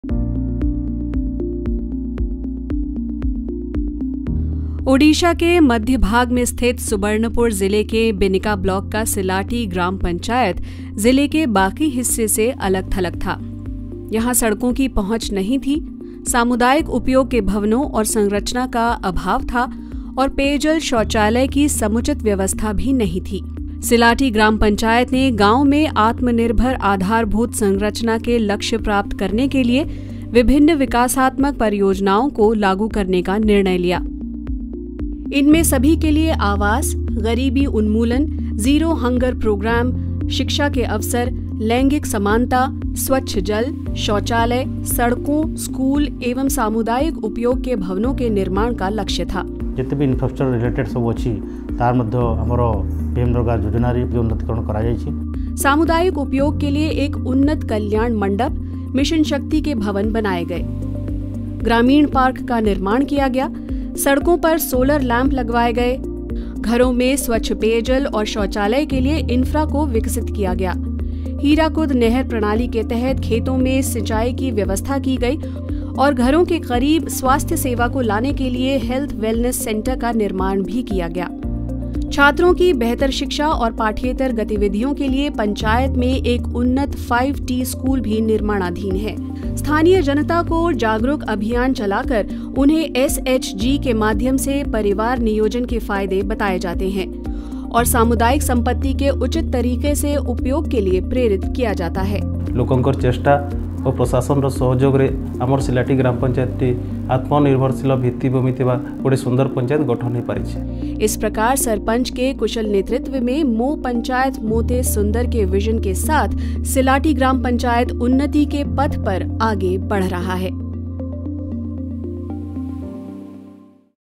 Odisha | Silati Gram Panchayat | Best Practices 2023 - PwGG
Odisha, Silati Gram Panchayat best practices under the LSDG Theme 6 - Self Sufficient Infrastructure in Panchayat.